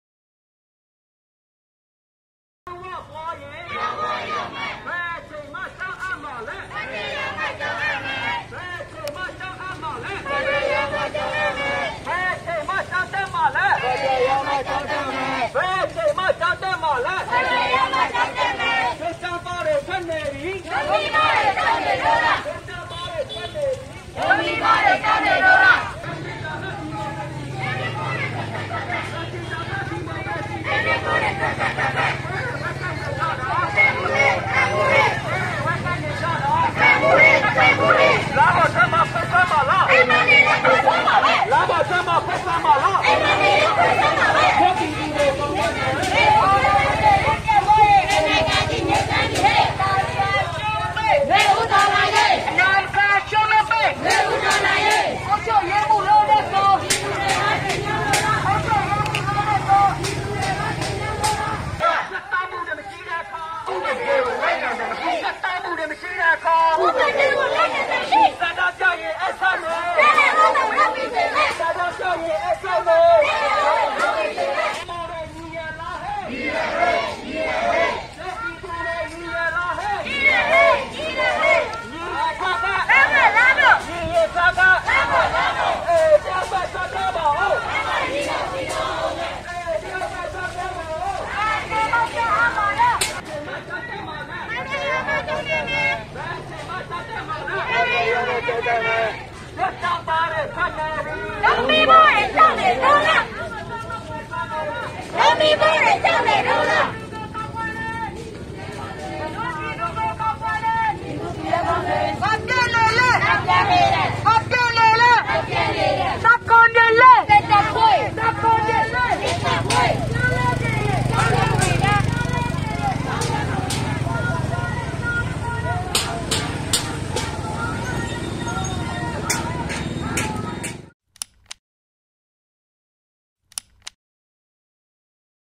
တစ်ရက်မပျက် ညင်းမော်ရွာသပိတ်
လောင်းလုံးမြိုနယ်ရှိ ညင်းမော်ရွာသပိတ်စစ်ကြောင်းဟာ မေလ ၂၉ ရက် မနက်ပိုင်းက စစ်အာဏာရှင်ဆန့်ကျင်ရေး ဆန္ဒပြခဲ့ပါတယ်။

စစ်အာဏာရှင်စနစ်ကို နေ့တိုင်းနီးပါးဆန့်ကျင်နေတာလည်း ဖြစ်ပါတယ်